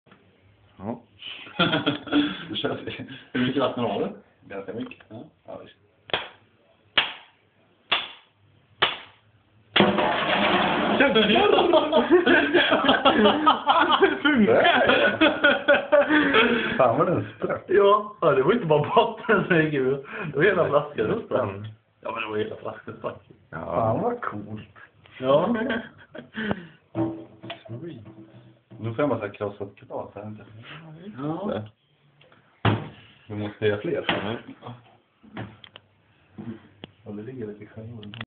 Bottle break
hit the top of the bottle, and the bottom will break.